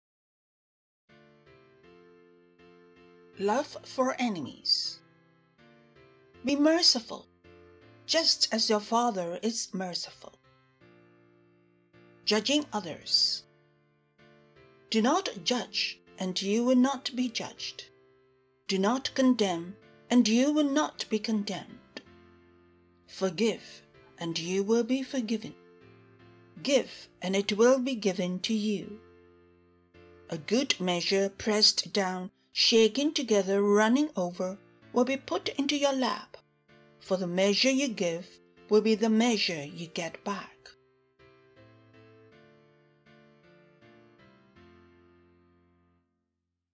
Luke 6:36-38 (Love for Enemies, Judging Others)
Luke 6:36-38
Love for Enemies

Be merciful, just as your Father is merciful.

Judging Others

‘Do not judge, and you will not be judged; do not condemn, and you will not be condemned. Forgive, and you will be forgiven; give, and it will be given to you. A good measure, pressed down, shaken together, running over, will be put into your lap; for the measure you give will be the measure you get back.’